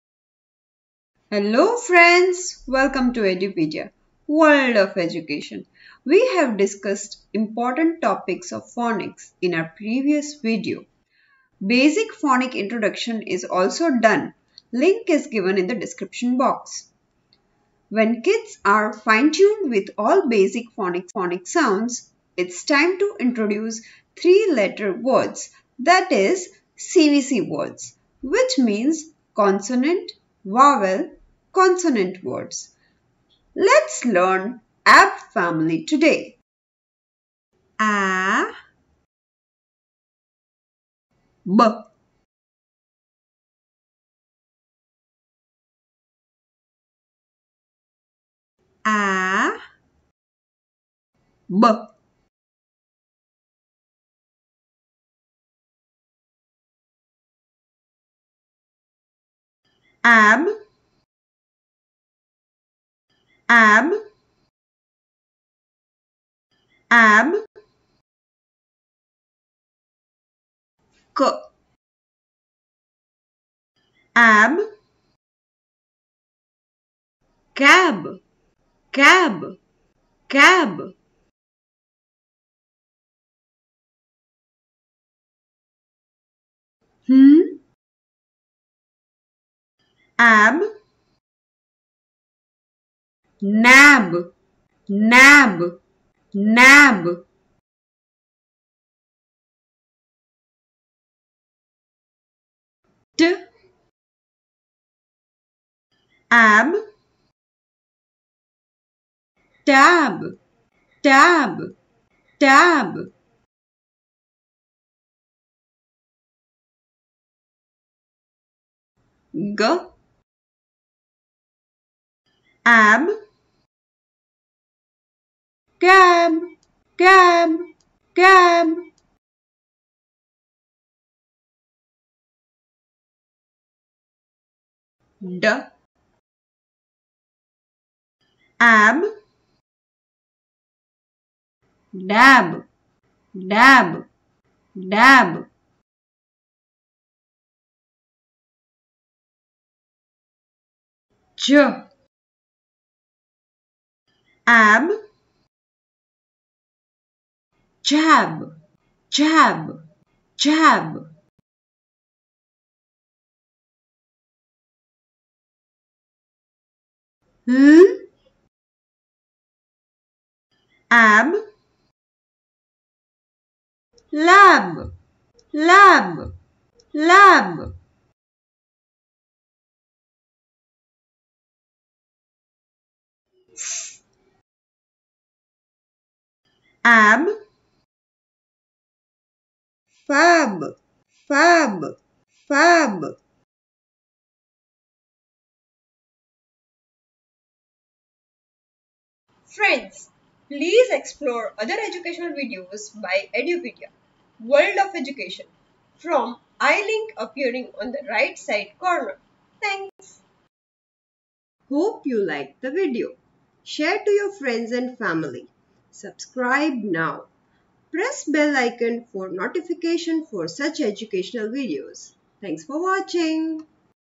Learn CVC Words - ab Family | English | Phonics | Sound
This Video will cover Phonics CVC Words - ab Family.  Three Letter ab Family Words.

Practice Quiz on How to use s | es | ies | ves. Singular to Plural.

Basic Concept of How to use This | That | These | Those in day to day use in English Language.